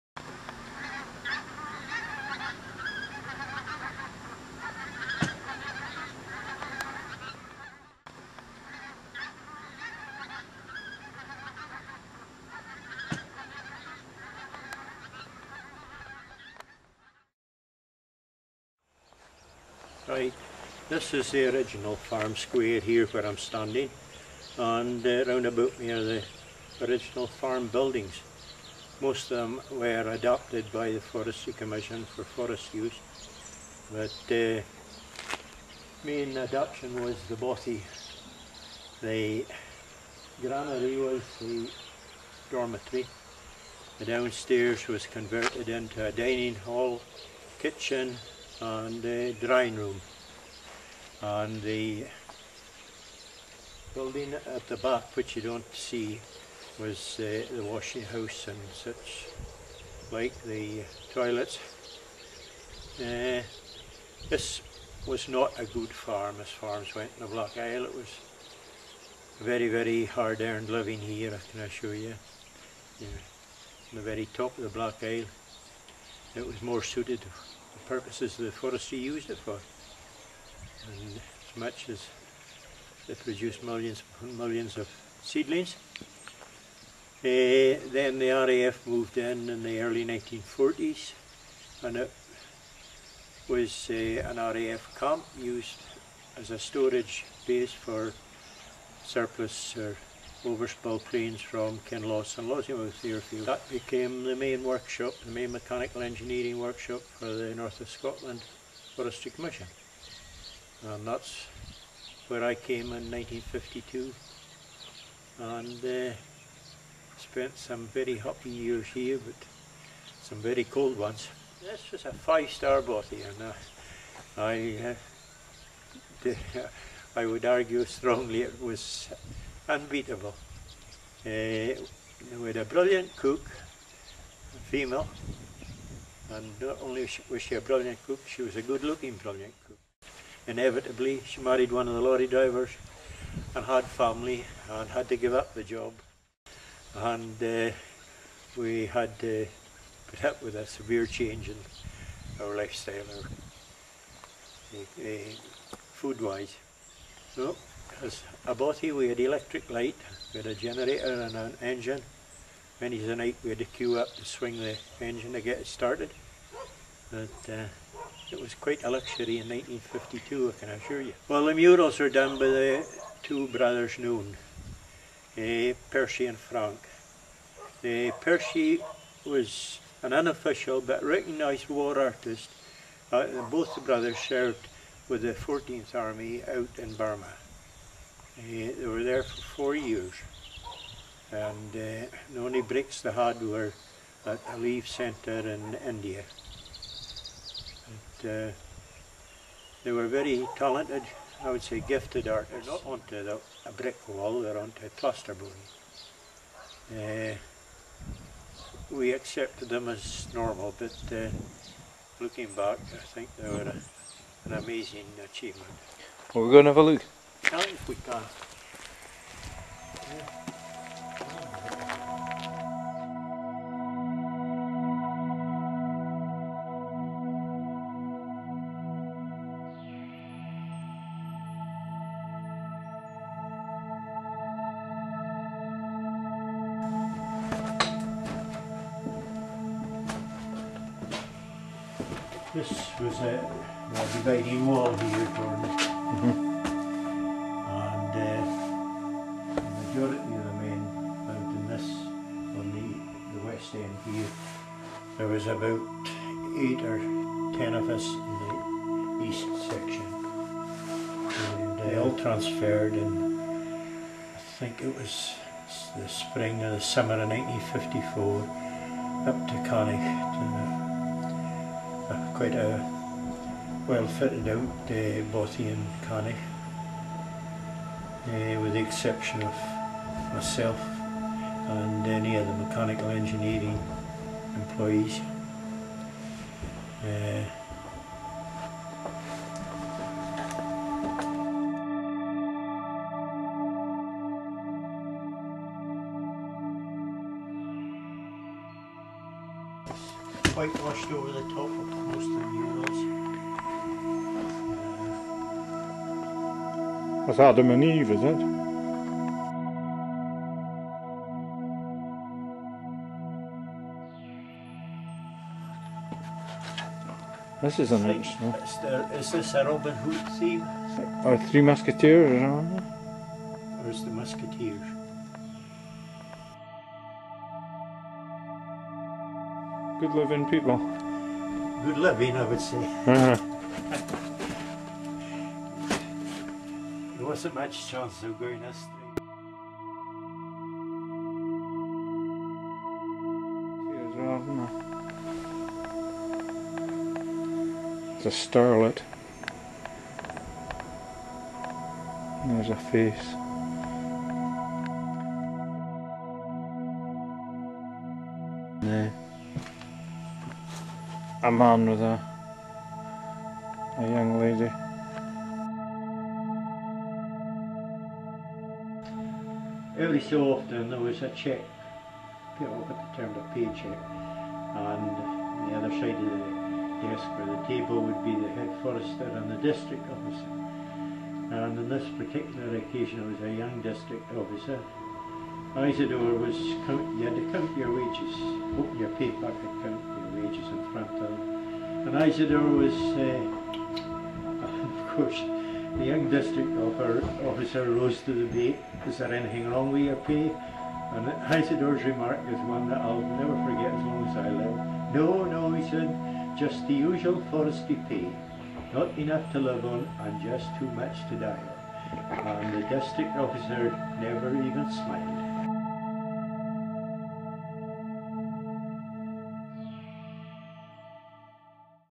Billy McKay at blackstand.wmv
Short video of Billy McKay at Blackstand Bothy. Billy talks about the history of Blackstand, life at the Bothy in its heyday and goes on to show us the unusual grafitti art done by the residents there.